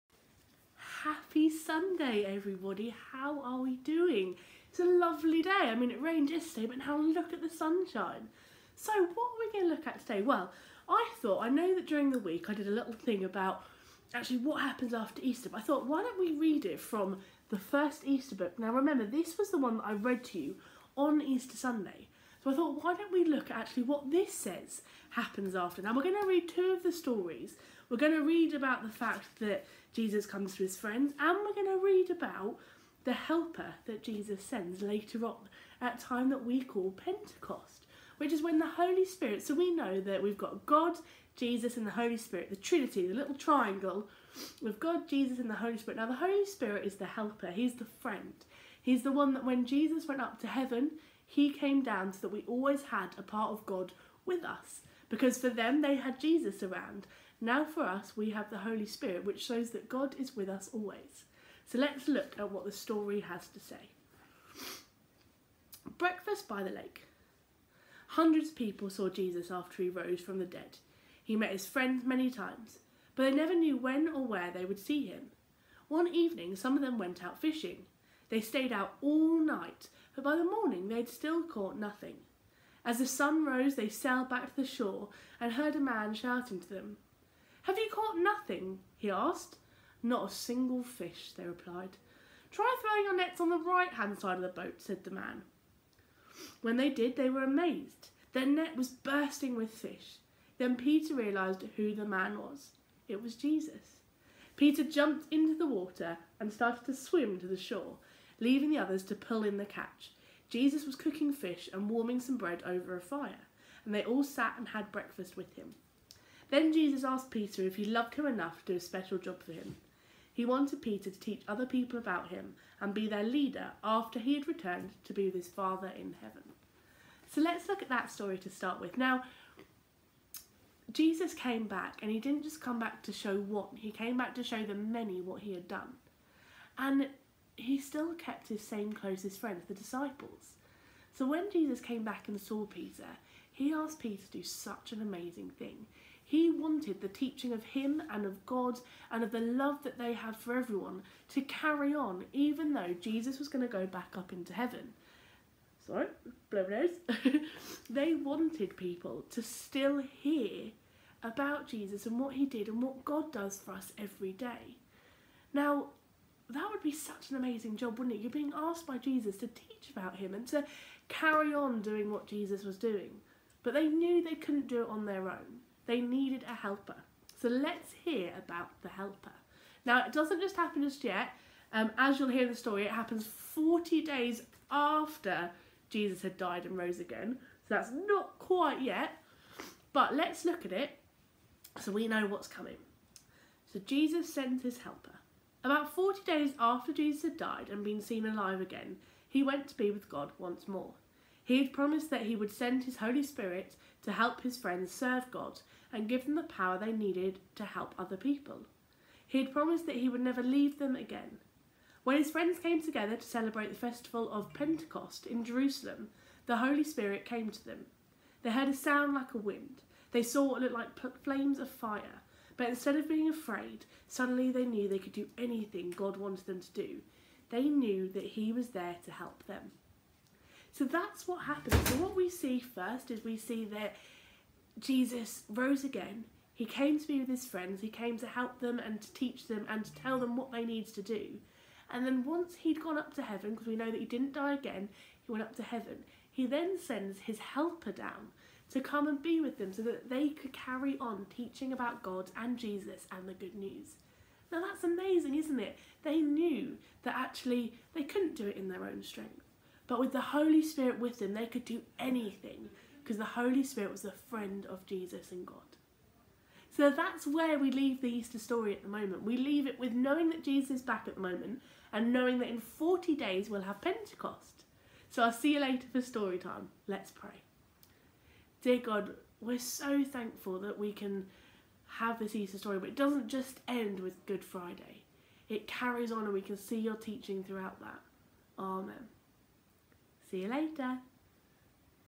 Happy Sunday, what happened next?
Come and join us as we look back on the Easter story and see what Jesus asked his friends to do, and who he sent to help.